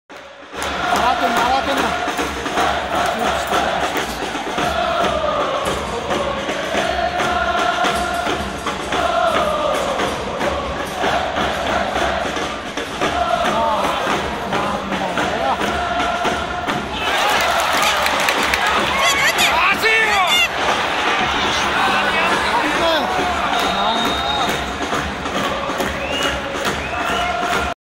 鹿島の攻撃 鹿島アントラーズ VS 水原三星 ACL準決勝
ACL準決勝1stレグ
鹿島アントラーズ 3-2 水原三星
Kashima Antlers 3-2 Suwon Samsung Bluewings